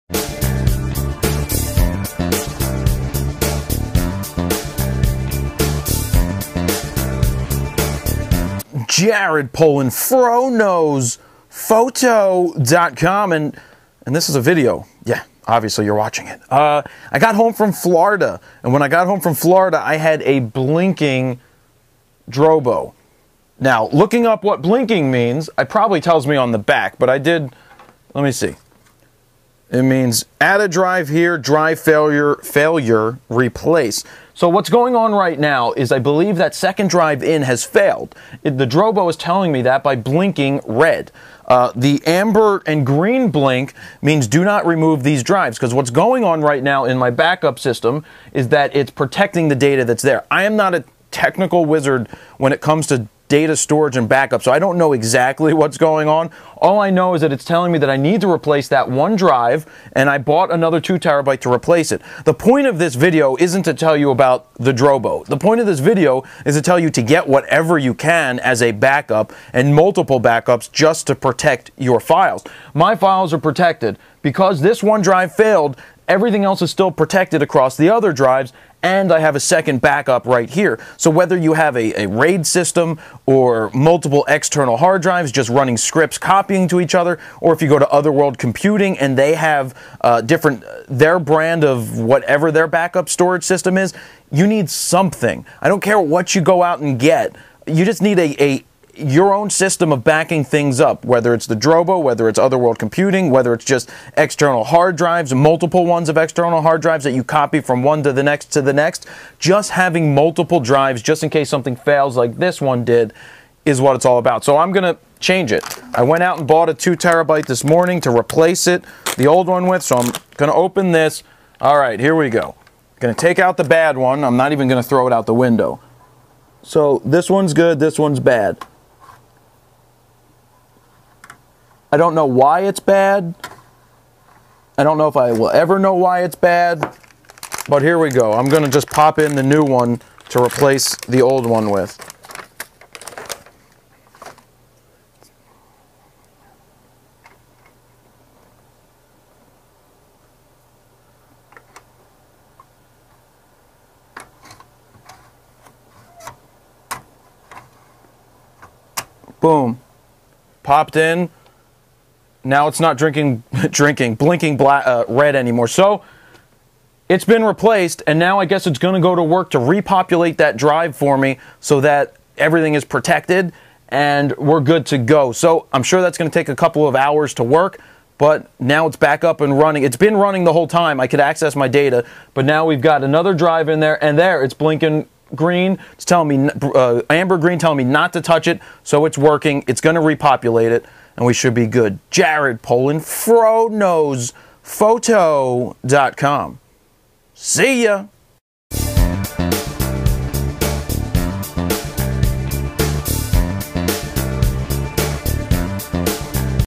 Protecting your DATA with Redundancy - Backup Solutions
One thing every photographer worries about or should be worrying about is protecting your photos for the long term.  You all know that I use Drobo who also sponsors some of the videos on the site.

This video and post is not about buying a Drobo it is about protecting your data with redundancy any way possible.

The reason this is coming up now is I came home from FLA to my DROBO telling me I had a drive go bad.  Now even though the one drive failed all of my data was still safe on the other 4 drives in the DROBO.  All I had to do was pop out the bad drive and replace it with a new drive and within ten minutes the drive was update and ready to go.

It is very important that you protect your photos anyway possible.  If this means going to OtherWorldComputing.com and purchasing one of their RAID set ups or simply having multiple harddrives on your desk just do something.  As we have been told for ever, its not a matter of if a drive fails but when, best off being prepared.